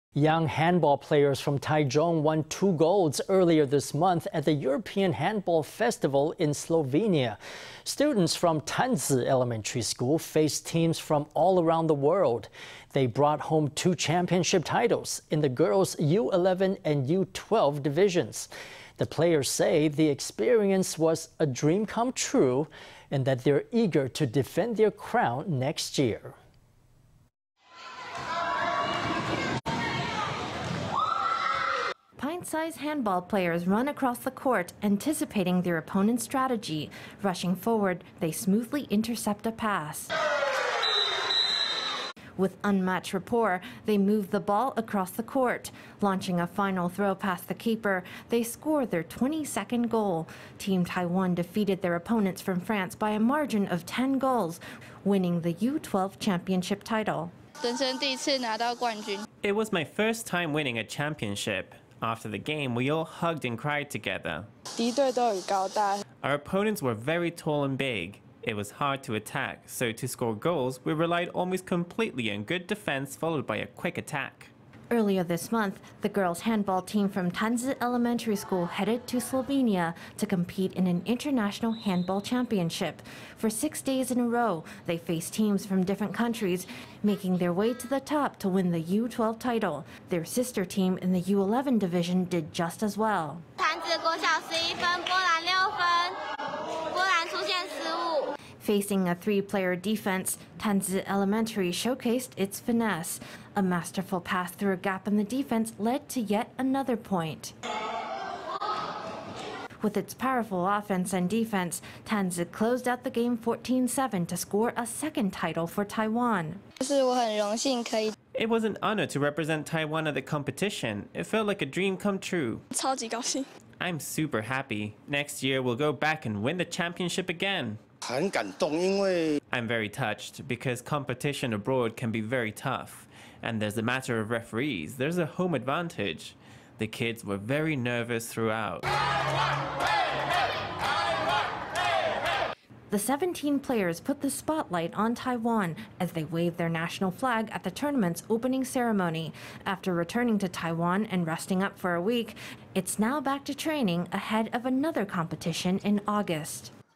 Taichung students win U-11 and U-12 handball titles in Slovenia
Young handball players from Taichung won two golds earlier this month at the European Handball Festival in Slovenia. Students from Tanzi Elementary School faced teams from all around the world. They brought home two championship titles, in the girls’ U-11 and U-12 divisions. The players say the experience was a dream come true, and that they’re eager to defend their crown next year.

Pint-sized handball players run across the court, anticipating their opponent’s strategy. Rushing forward, they smoothly intercept a pass.

With unmatched rapport, they move the ball across the court. Launching a final throw past the keeper, they score their 22nd goal. Team Taiwan defeated their opponents from France by a margin of 10 goals, winning the U-12 championship title.

U-12 handball player
Tanzi Elementary School
It was my first time winning a championship. After the game, we all hugged and cried together.

U-12 handball player
Tanzi Elementary School
Our opponents were very tall and big. It was hard to attack, so to score goals, we relied almost completely on good defense followed by a quick attack.

Earlier this month, the girls’ handball team from Tanzi Elementary School headed to Slovenia to compete in an international handball championship. For six days in a row, they faced teams from different countries, making their way to the top to win the U-12 title. Their sister team in the U-11 division did just as well.

Facing a three-player defense, Tanzi Elementary showcased its finesse. A masterful pass through a gap in the defense led to yet another point.

With its powerful offense and defense, Tanzi closed out the game 14:7 to score a second title for Taiwan.

U-11 handball player
Tanzi Elementary School
It was an honor to represent Taiwan at the competition. It felt like a dream come true.

U-11 handball player
Tanzi Elementary School
I’m super happy. Next year, we’ll go back and win the championship again.

Chen Ming-chen
Tanzi Elementary School principal
I’m very touched, because competition abroad can be very tough. And there’s the matter of referees, there’s a home advantage. The kids were very nervous throughout.

The 17 players put the spotlight on Taiwan, as they waved their national flag at the tournament’s opening ceremony. After returning to Taiwan and resting up for a week, it’s now back to training ahead of another competition in August.